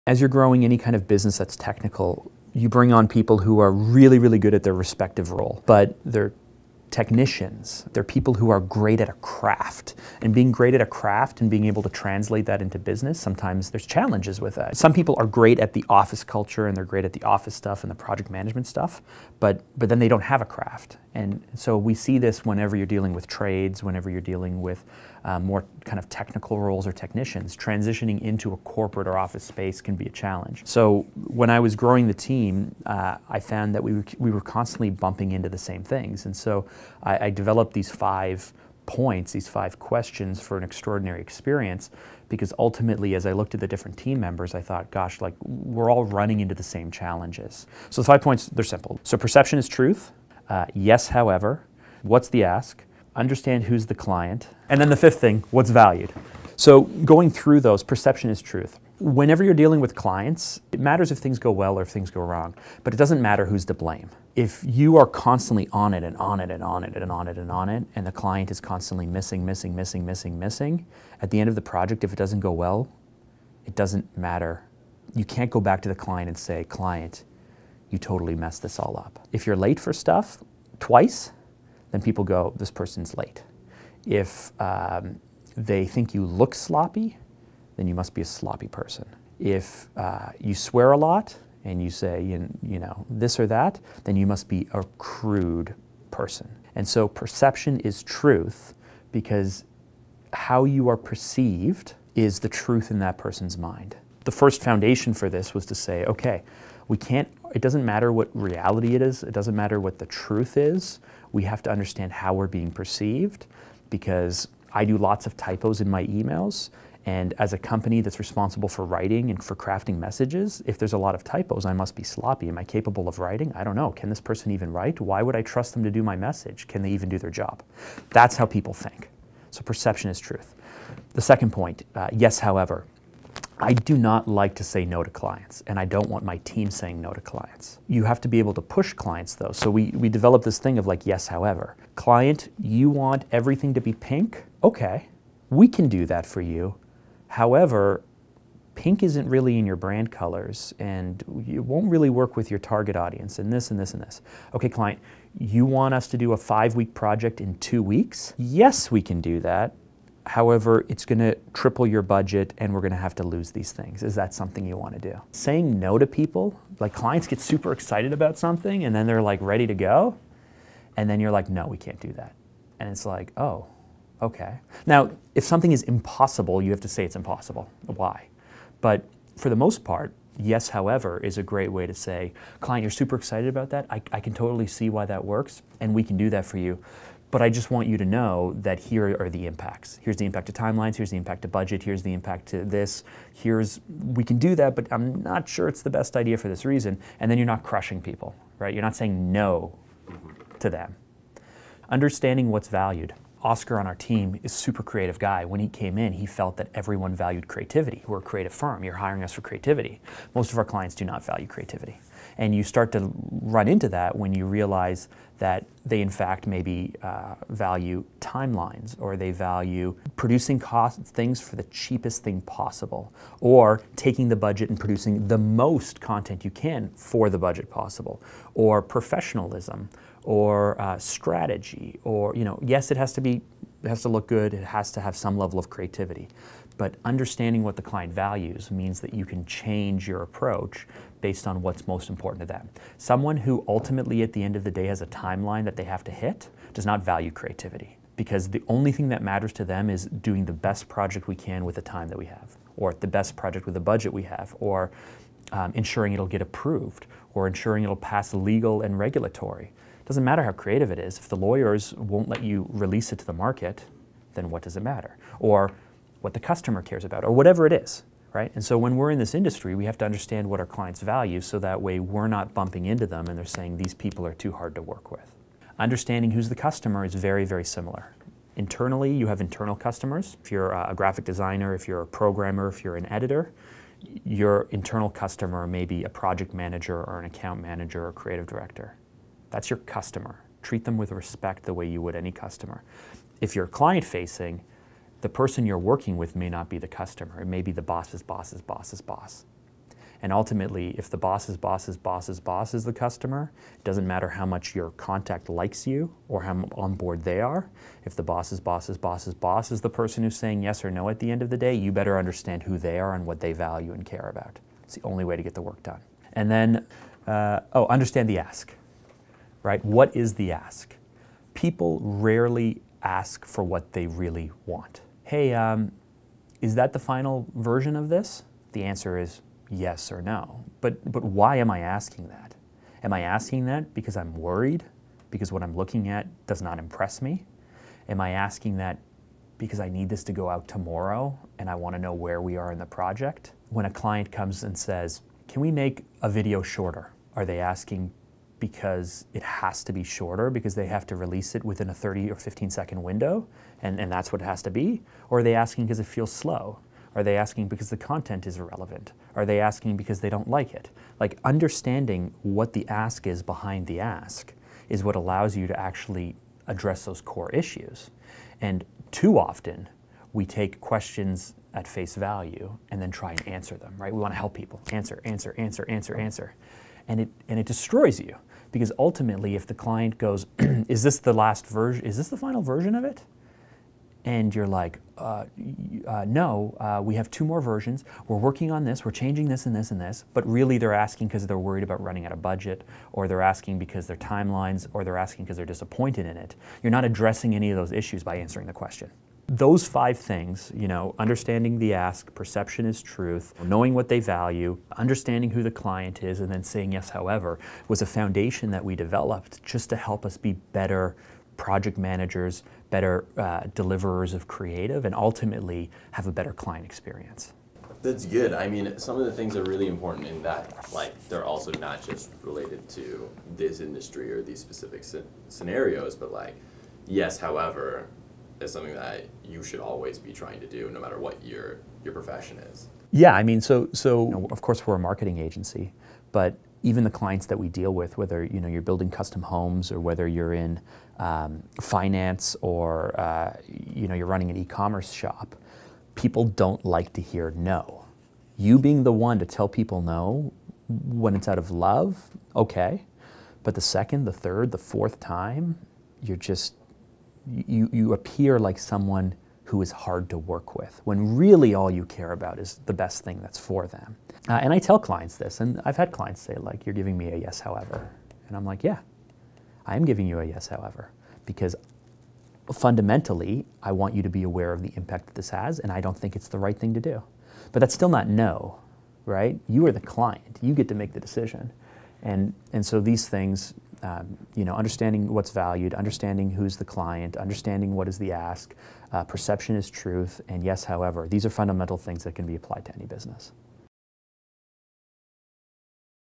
Delivering Extraordinary Client Experiences | The Building It Series
Each week we release a new episode of The Building It Series where we share insight, behind the scene thinking, and what it takes to build an extraordinary business.

In this episode, Mark talks about delivering extraordinary client experience and explains the 5 areas of focus he developed for his team at PHANTA.

Those five points again are:

Perception is truth.
Yes, however.
Who is the customer?
What do they value?
What is the ask?